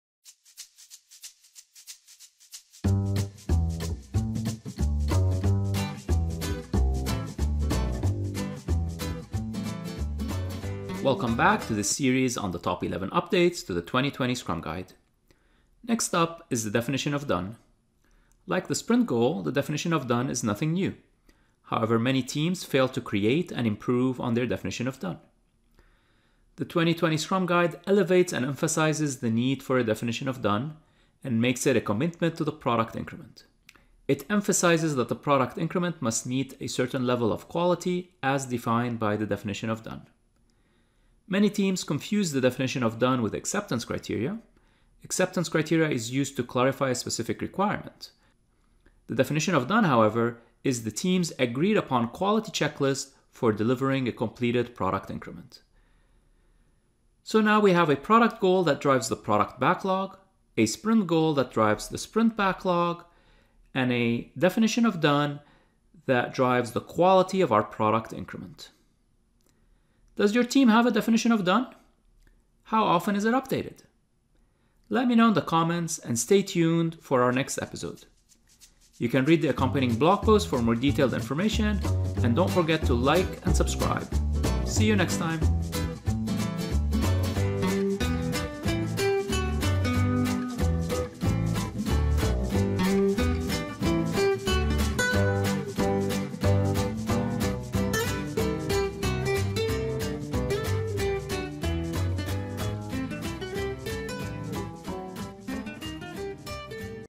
Top 11 Updates to the 2020 Scrum Guide - Definition of Done - Episode 7
Welcome back to this series on the top 11 updates to the 2020 Scrum guide. Next up is the Definition of Done.

Does your team have a definition of done? How often is it updated?